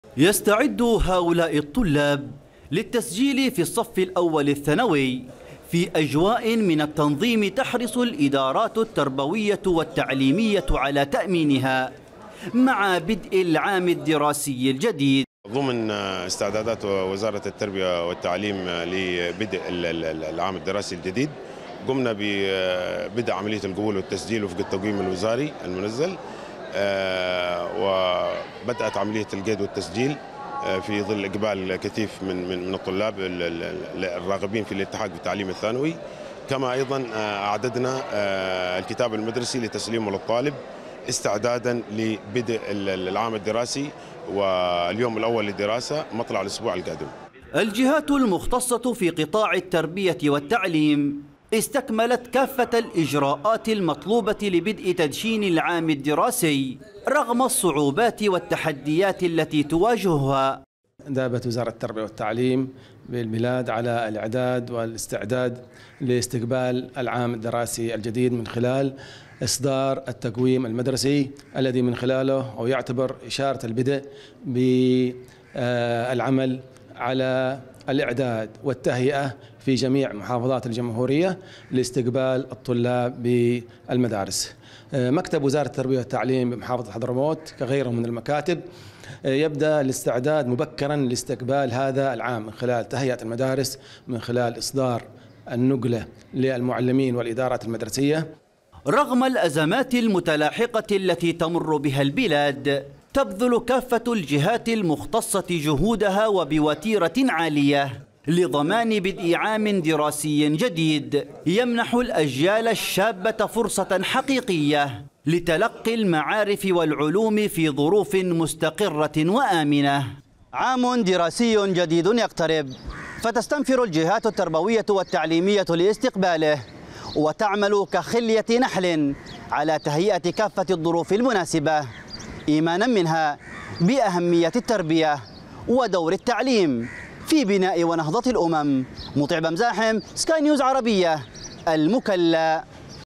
اليمن.. استعدادات مكثفة لاستقبال العام الدراسي الجديد
تستعد وزارة التربية والتعليم اليمنية لاستقبال العام الدراسي الجديد.
ويقول مسؤولو قطاع التربية، إنهم يبذلون جهودا مكثفة لتأمين عام دراسي آمن للطلاب، في ظل الأزمات التي يعيشها اليمن.